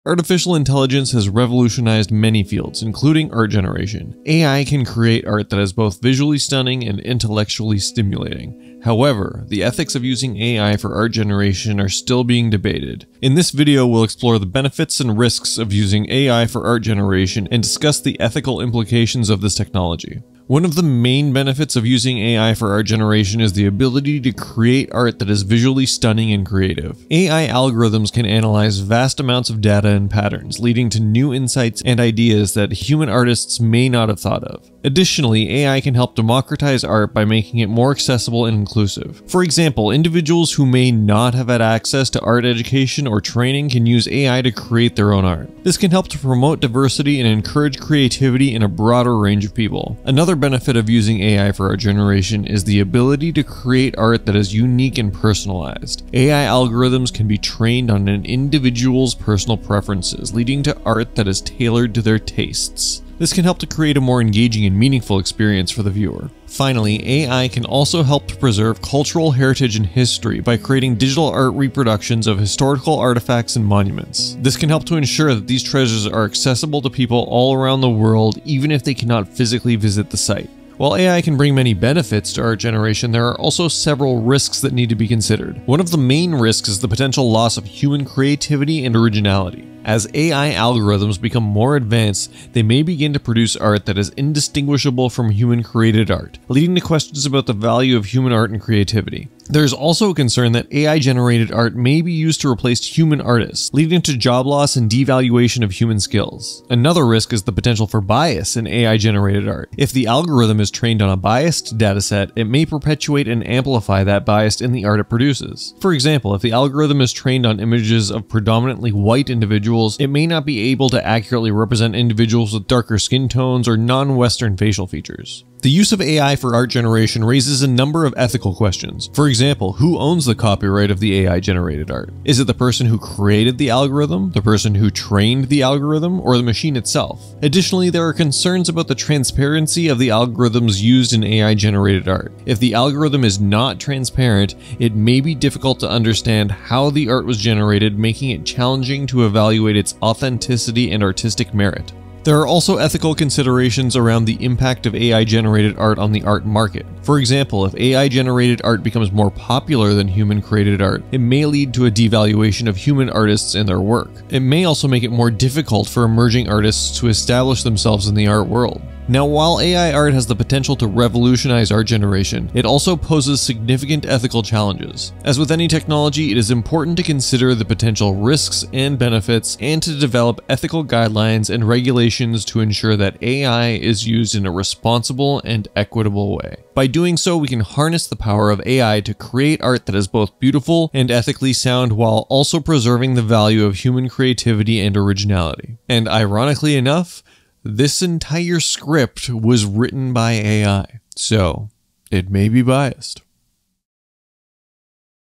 The Ethical Dilemma of AI Generated Art
In recent years, artificial intelligence has made impressive strides in generating art that is often indistinguishable from human-made art. While the technology is fascinating, it also raises some significant ethical concerns. In this video, we explore the pros and cons of AI-generated art, examining its potential benefits and drawbacks. We delve into the ethical dilemmas surrounding AI art, including questions of authorship, creativity, and authenticity. As AI-generated art becomes more prevalent, it's essential that we consider the ethical implications and make informed decisions about its use. Join us as we navigate this complex and evolving field and try to make sense of the ethical dilemmas of AI-generated art. Stay classy Internet.